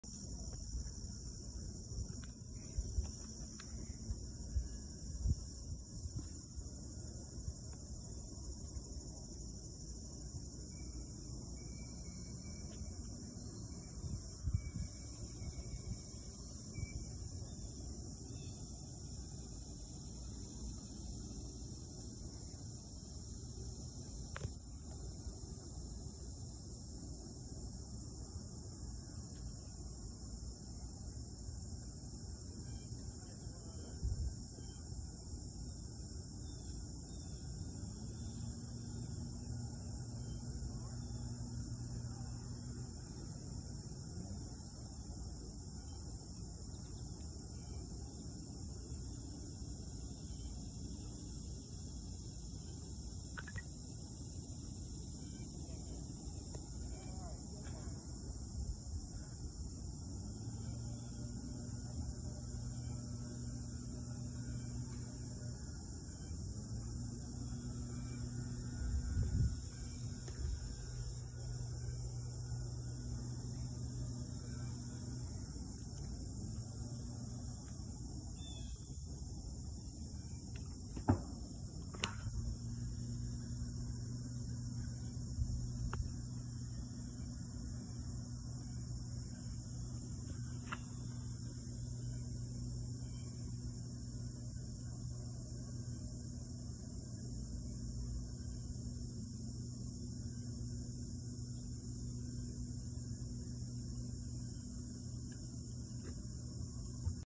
Larger snapping turtle failing to yield right of way
Two snapping turtles approach head on and one decides discretion is the better part of valor and decides to take the waterway roundabout.